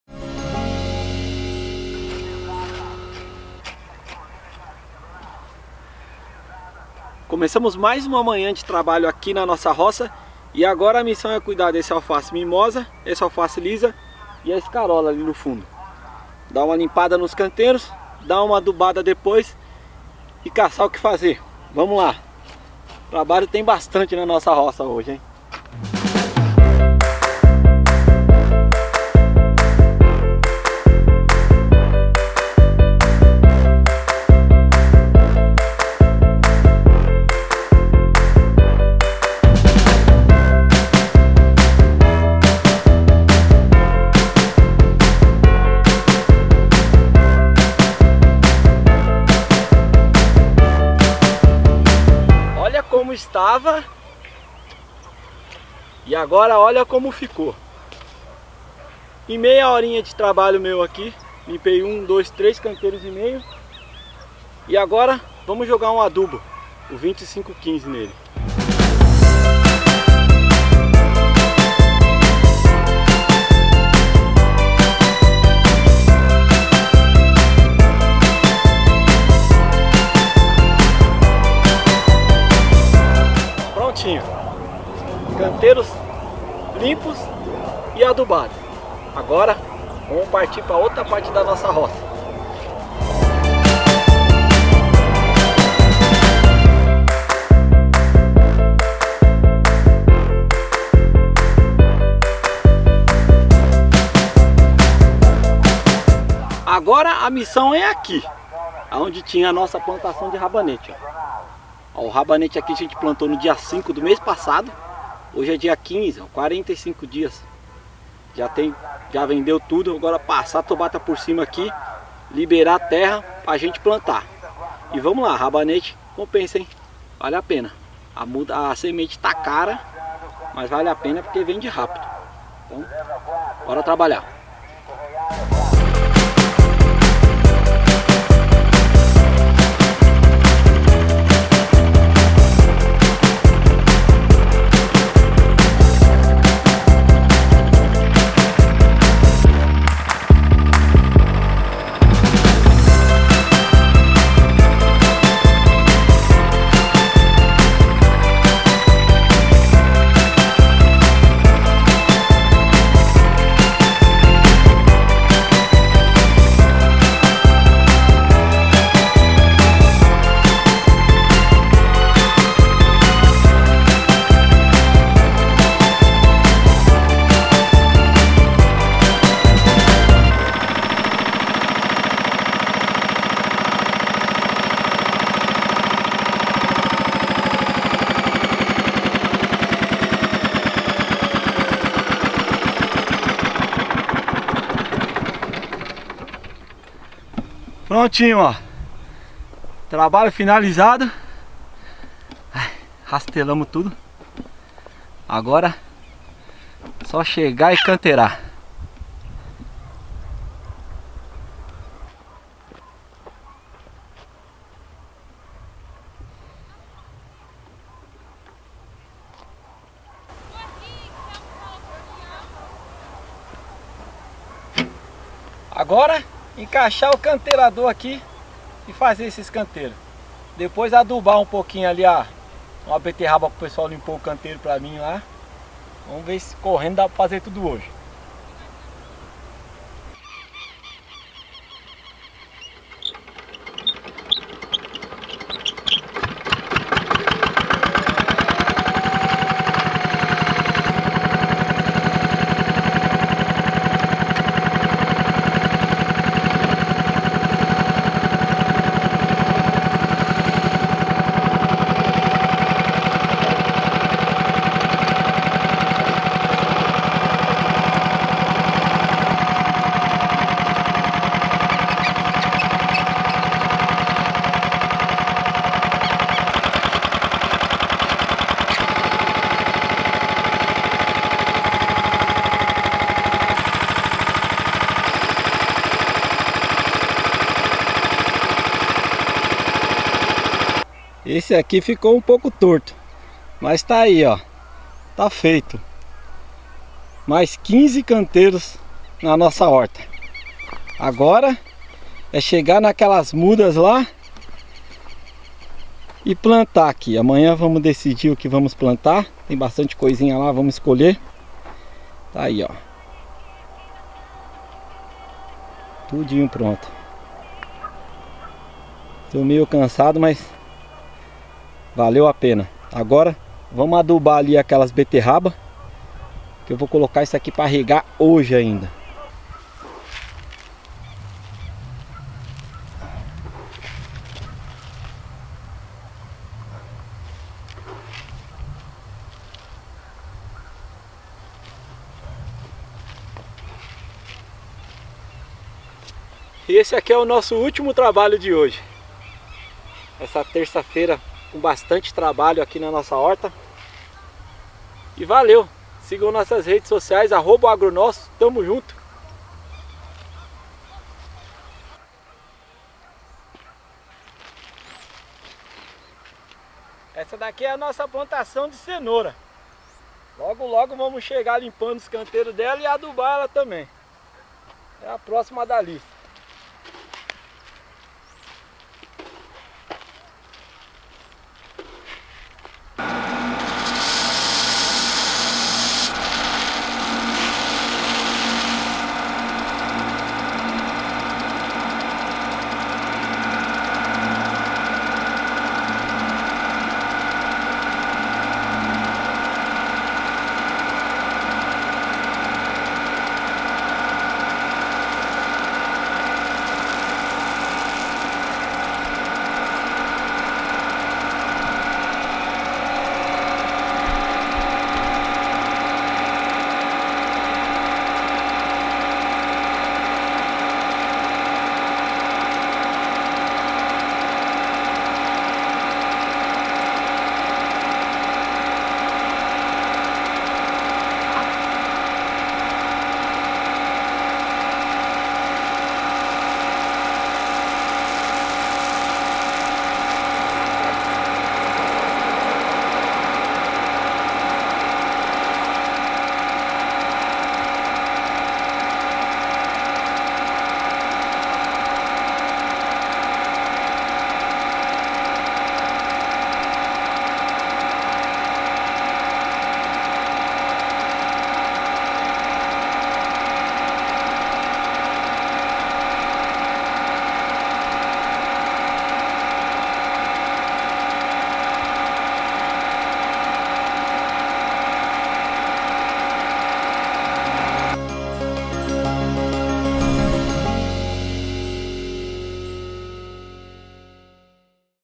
TRABALHO NA ROÇA É ASSIM, LIMPAR CANTEIRO PASSAR TRATOR TOBATA E ADUBAR
Dia de muito trabalho na horta do O Agro Nosso.
Lá Marcos Meira limpou conteiro, adubou alface, passou o micro trator tobata e por fim regou a horta.